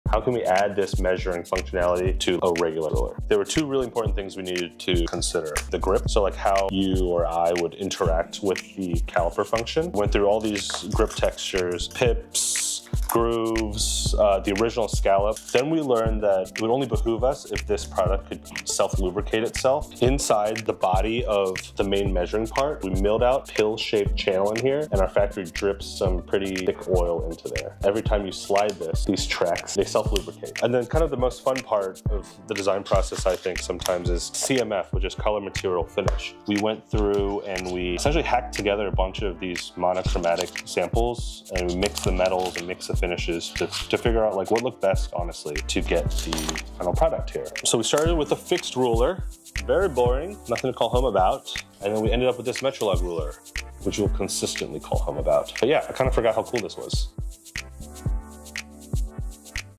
How we added caliper functionality to a sleek straight edge ruler.
Explore the Metrolog Ruler in detail below.